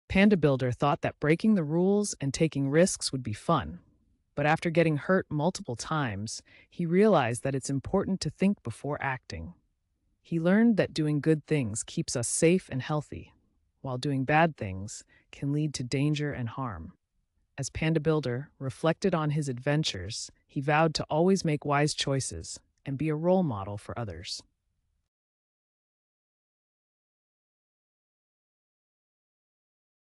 The Adventures of Panda Builder
Follow Panda Builder as he learns that taking part in unacceptable or dangerous activities without a good reason can lead to injury or illness.